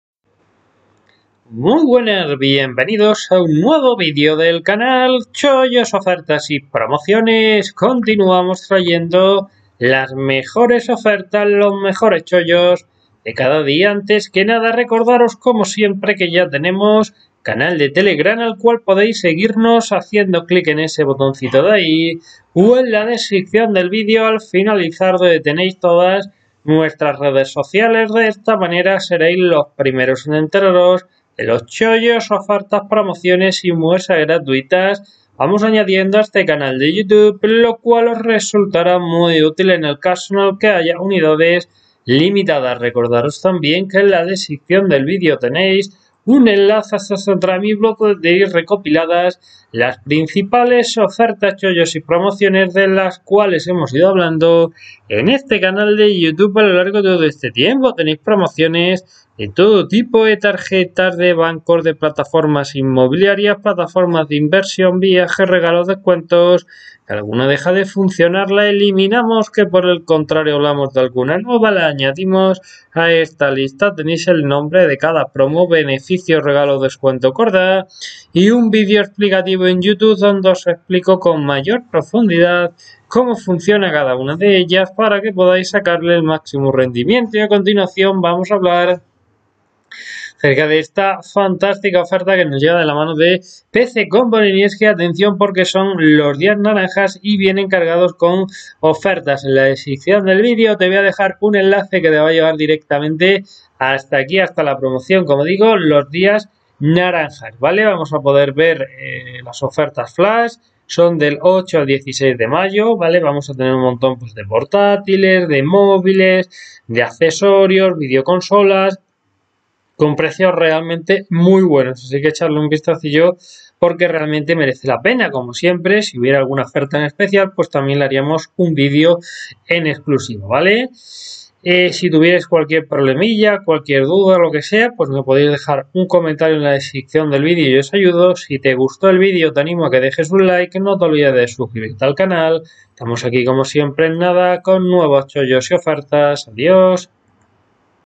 🔥 ¡Días Naranjas en PcComponentes del 8 al 16 de Mayo 2023! Ofertas, Cupones y Descuentos 🔥
🔥 ¡Días Naranjas en PcComponentes del 8 al 16 de Mayo 2023!

Días Naranjas 2022
¡Vuelven los Días Naranjas a PcComponentes! para alegrarte el otoño. Prepárate para disfrutar de una semana llena de ofertas en más de 300 productos. Del 8 al 16 de Mayo tienes una cita con la semana más naranja de PcComponentes.